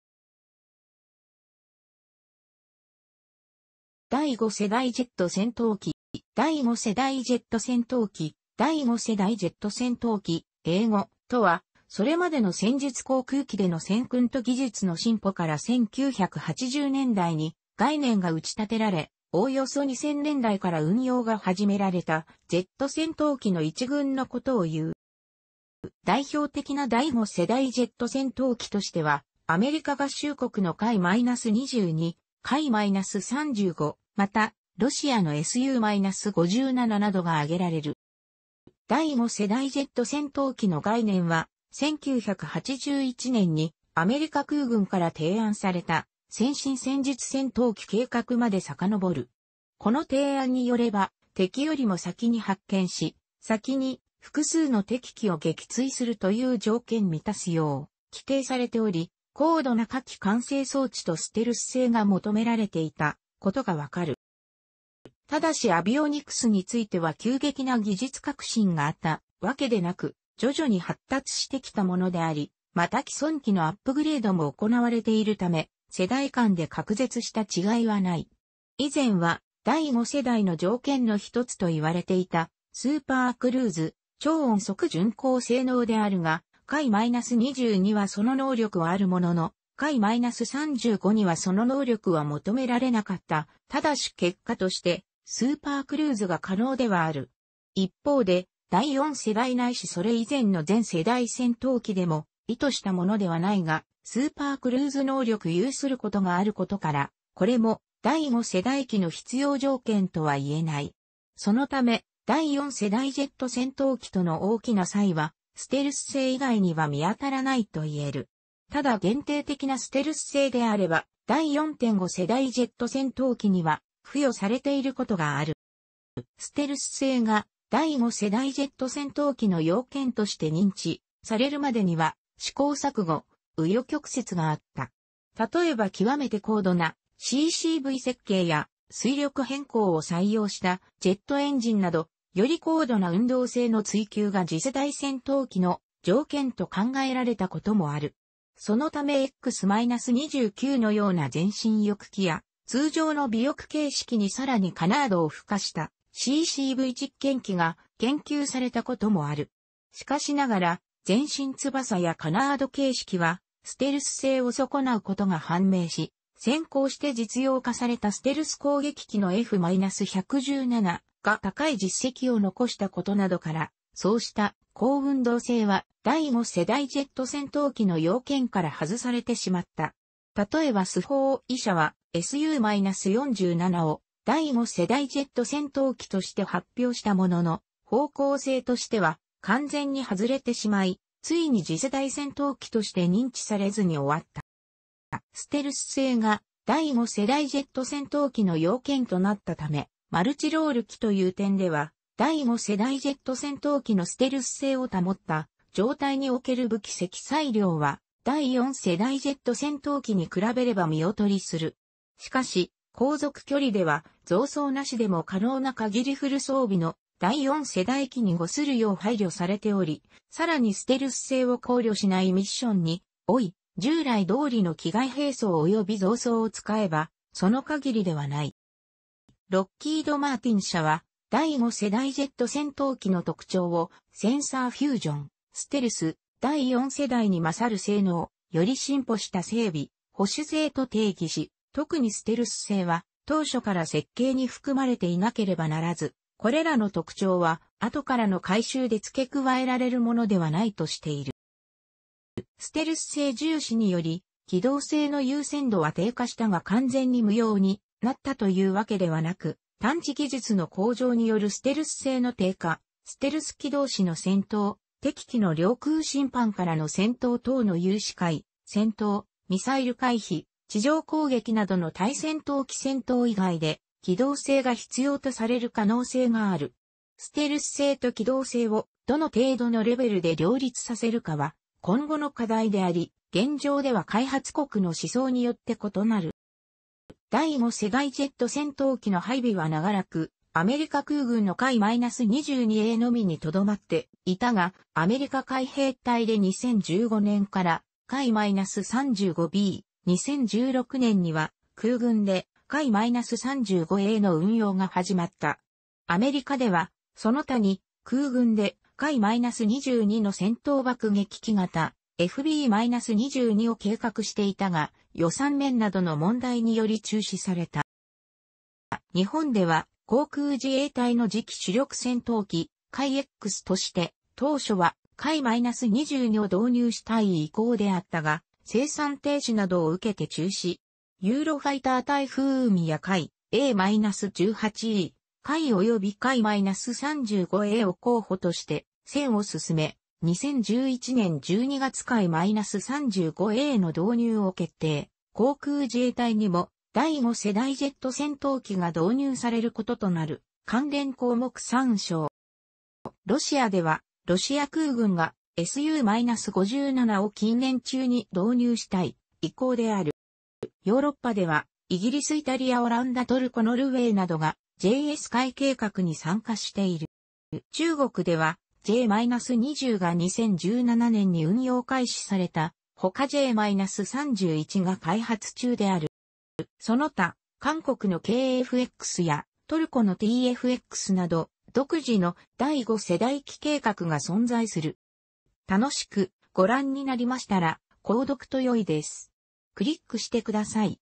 第5世代ジェット戦闘機
第5世代ジェット戦闘機（だい5せだいジェットせんとうき、英語:）とは、それまでの戦術航空機での戦訓と技術の進歩から1980年代に概念が打ち立てられ、おおよそ2000年代から運用が始められたジェット戦闘機の一群のことをいう。

代表的な第5世代ジェット戦闘機(計画段階も含めて)としては、アメリカ合衆国のF-22、F-35、またロシアのSu-57などが挙げられる。

第5世代ジェット戦闘機の概念は、1981年にアメリカ空軍から提案された先進戦術戦闘機計画まで遡る。この提案によれば、「敵よりも先に発見し、先に（複数の敵機を）撃墜する」という条件を満たすよう規定されており、高度な火器管制装置とステルス性が求められていたことがわかる。

ただしアビオニクスについては急激な技術革新があったわけでなく、徐々に発達してきたものであり、また既存機のアップグレードも行われているため、世代間で隔絶した違いは無い。以前は第5世代の条件の一つと言われていたスーパークルーズ（超音速巡航）性能であるが、F-22はその能力はあるものの、F-35にはその能力は求められなかった（ただし結果としてスーパークルーズが可能ではある）。一方で、第4世代ないしそれ以前の前世代戦闘機でも意図したものではないがスーパークルーズ能力を有することがあることから、これも第5世代機の必要条件とは言えない。そのため第4世代ジェット戦闘機との大きな差異はステルス性以外には見当たらないといえる。ただ限定的なステルス性であれば、第4.5世代ジェット戦闘機には付与されていることがある。

ステルス性が第5世代ジェット戦闘機の要件として認知されるまでには、試行錯誤、紆余曲折があった。例えば極めて高度なCCV設計や、推力偏向を採用したジェットエンジンなど、より高度な運動性の追求が次世代戦闘機の条件と考えられた事もある。そのためX-29のような前進翼機や、通常の尾翼形式にさらにカナードを付加したCCV実験機が、研究されたこともある。しかしながら前進翼やカナード形式はステルス性を損なうことが判明し、先行して実用化されたステルス攻撃機のF-117が高い実績を残したことなどから、そうした高運動性は第5世代ジェット戦闘機の要件から外されてしまった。例えばスホーイ社はSu-47を「第5世代ジェット戦闘機」として発表したものの、方向性としては完全に外れてしまい、ついに次世代戦闘機として認知されずに終わった。

ステルス性が第5世代ジェット戦闘機の要件となったため、マルチロール機という点では、第5世代ジェット戦闘機のステルス性を保った状態における武器積載量は、第4世代ジェット戦闘機に比べれば見劣りする。しかし航続距離では増槽なしでも可能な限りフル装備の第4世代機に伍するよう配慮されており、さらにステルス性を考慮しないミッションにおいて、従来どおりの機外兵装および増槽を使えば、その限りではない。

ロッキード・マーティン社は、第5世代ジェット戦闘機の特徴を「センサー・フュージョン」「ステルス」「（第4世代に勝る）性能」「より進歩した整備・保守性」と定義し、特にステルス性は当初から設計に含まれていなければならず、これらの特徴は後からの改修で付け加えられるものではないとしている。

ステルス性重視により機動性の優先度は低下したが完全に無用になったというわけではなく、探知技術の向上によるステルス性の低下、ステルス機同士の戦闘、敵機の領空侵犯からの戦闘等の有視界戦闘、ミサイル回避、地上攻撃などの対戦闘機戦闘以外で機動性が必要とされる可能性がある。ステルス性と機動性をどの程度のレベルで両立させるかは今後の課題であり、現状では開発国の思想によって異なる。

第5世代ジェット戦闘機の配備は長らくアメリカ空軍のF-22Aのみにとどまっていたが、アメリカ海兵隊で2015年からF-35B、2016年には空軍でF-35Aの運用が始まった。アメリカではその他に、空軍でF-22の戦闘爆撃機型、FB-22を計画していたが、予算面などの問題により中止された。

日本では航空自衛隊の次期主力戦闘機（F-X）として、当初はF-22を導入したい意向であったが、生産停止などを受けて中止。ユーロファイター タイフーンやF/A-18E/F及びF-35Aを候補として選考を進め、2011年12月F-35Aの導入を決定、航空自衛隊にも第5世代ジェット戦闘機が導入されることとなる（関連項目参照）。

ロシアではロシア空軍が、Su-5...